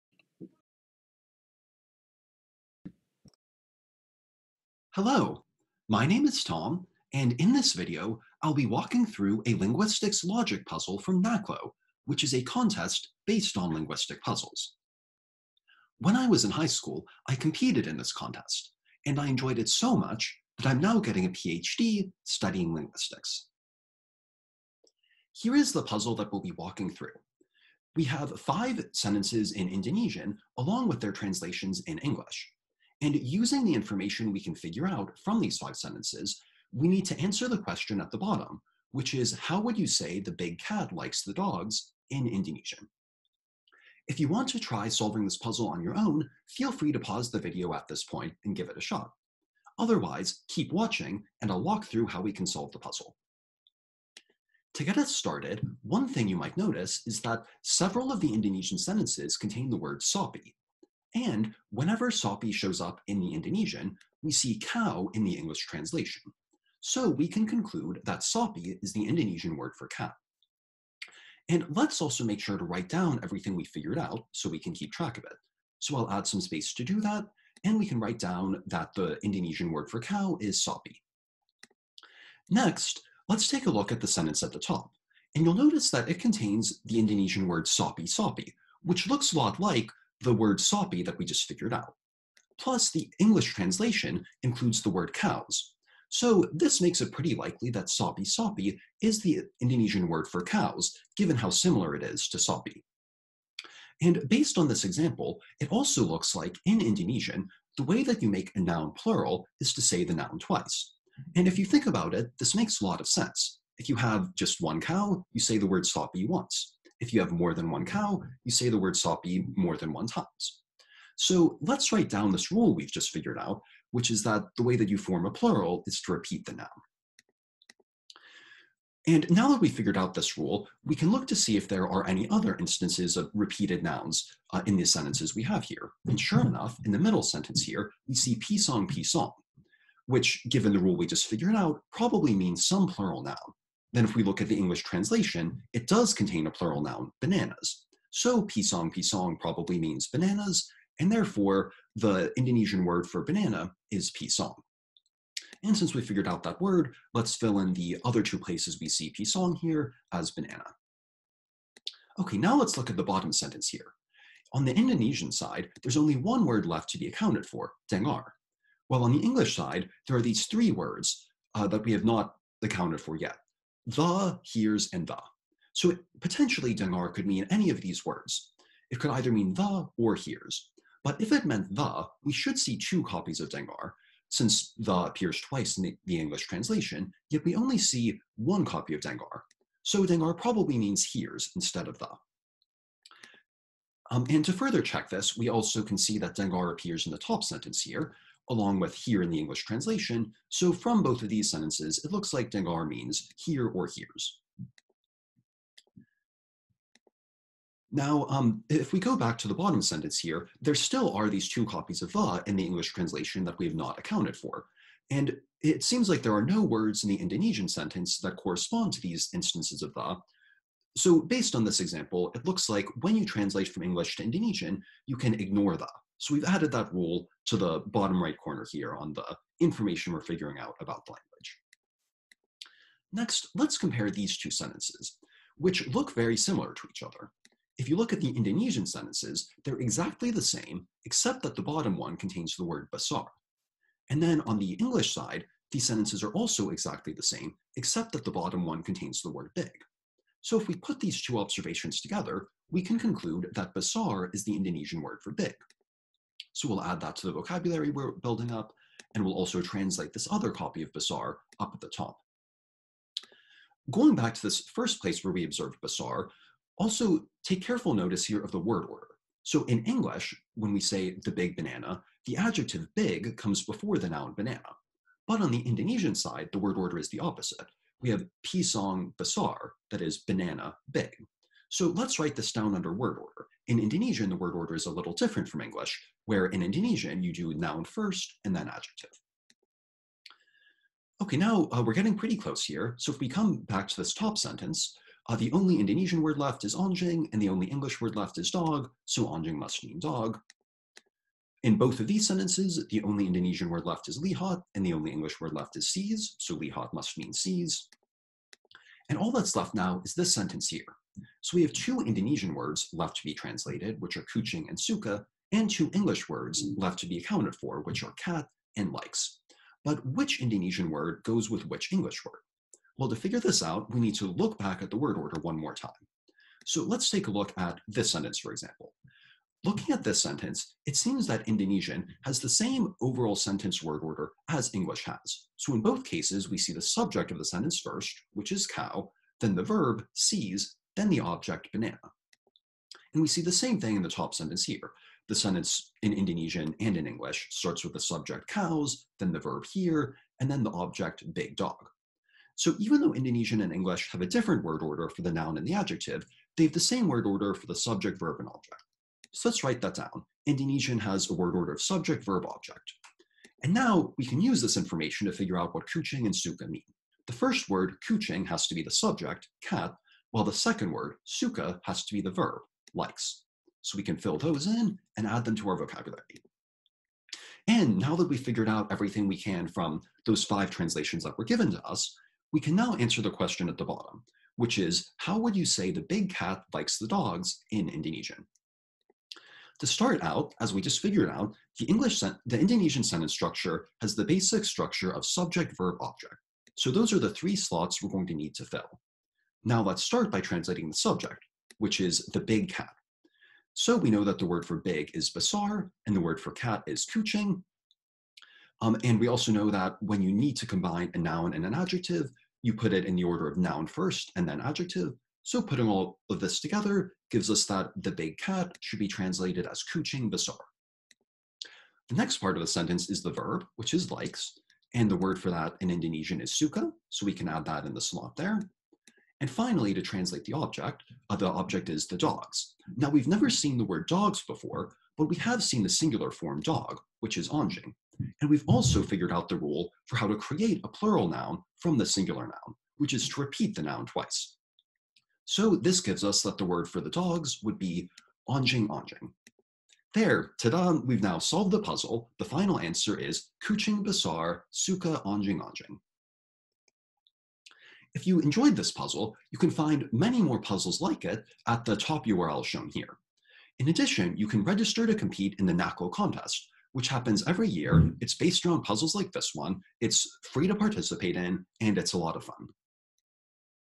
NACLO Problem Walkthrough: An Indonesian Enigma
Using only 5 examples, figure out how to translate an English sentence into Indonesian - like cracking the Rosetta Stone! In the process, we will discover some of the interesting ways in which Indonesian is similar to, and different from, English.

Narrated by Tom McCoy, who was drawn to the NACLO contest because he loved wordplay, logic puzzles, and cracking codes. Tom competed in NACLO during all 4 years of high school and, during his senior year, was on the US team that won first place in the world at the 2013 International Linguistics Olympiad in Manchester, UK. After high school, he majored in Linguistics at Yale and is now getting a PhD in Cognitive Science at Johns Hopkins, focusing on computational linguistics.